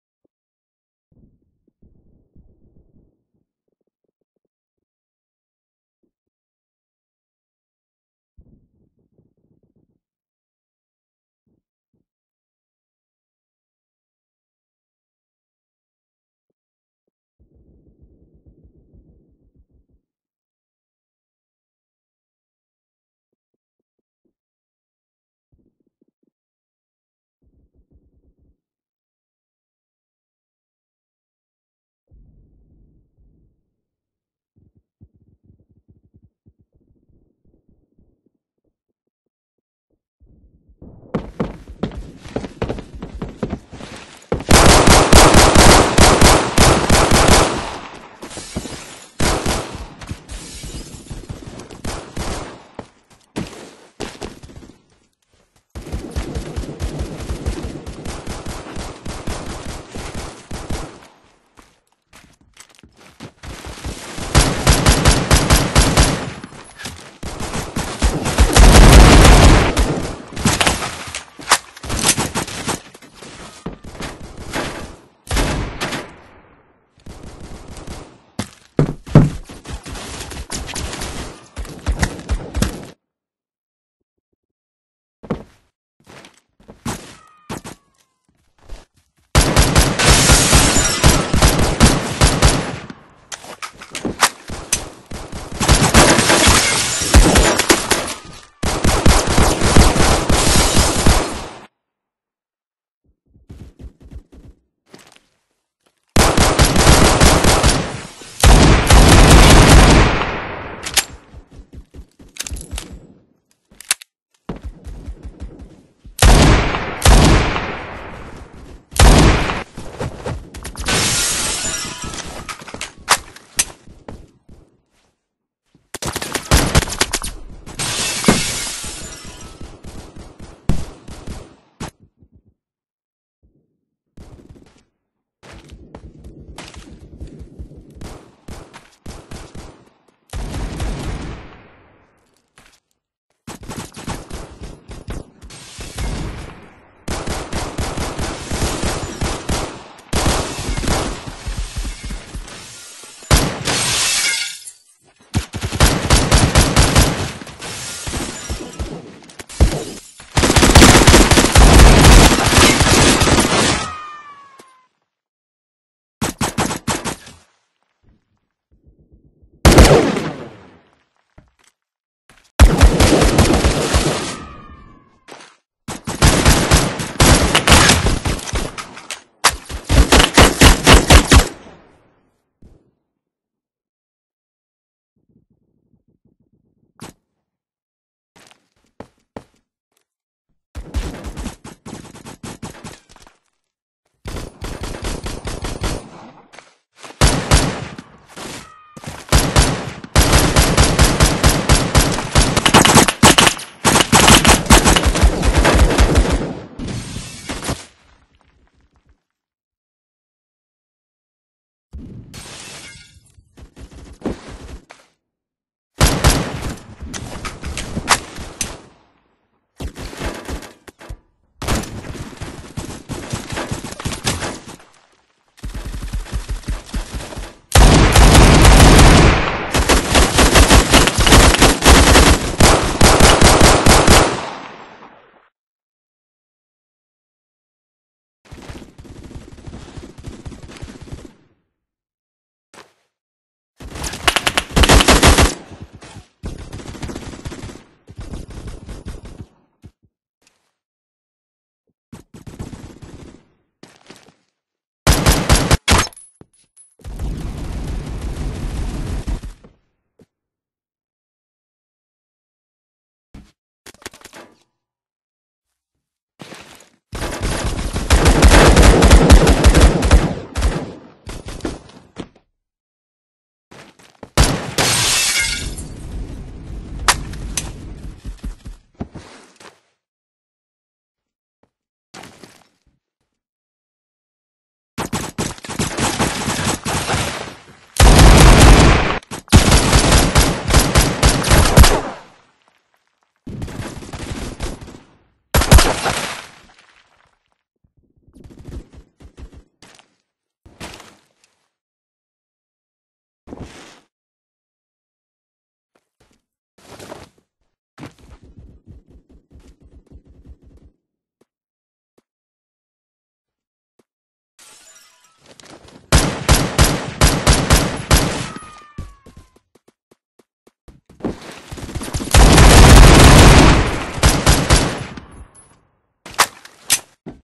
The "Catza" gun challenge
Why its called the "Catza" gun challenge is because these are some of the most precise guns in game :P sorry I died so many times tho...